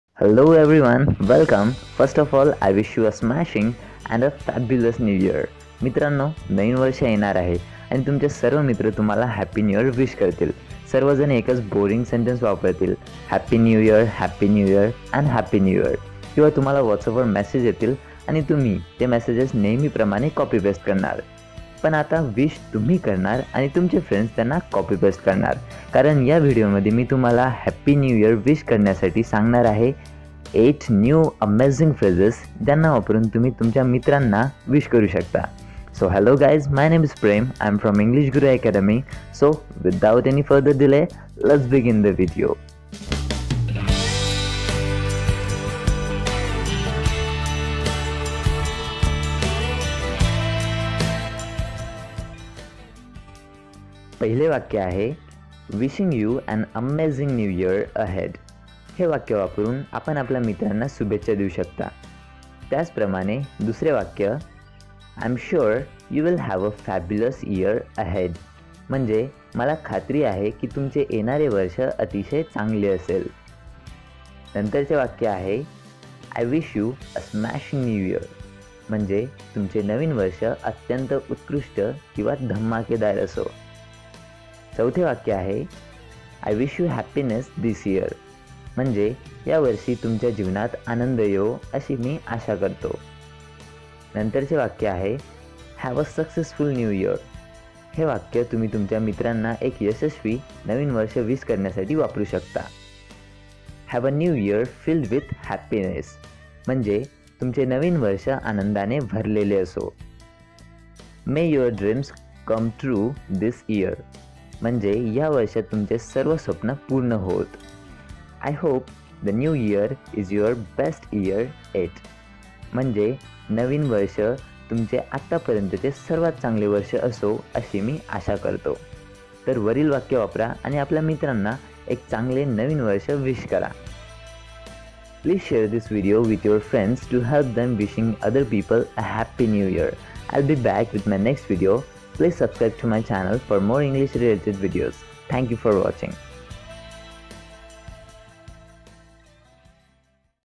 Wishing Happy New Year In English|नवीन वर्षाच्या शुभेच्छा देणे | English Sentences
This video will teach you to wish your friends Happy New Year in English... How To Wish Happy New Year In English... Use these sentences and make your impression...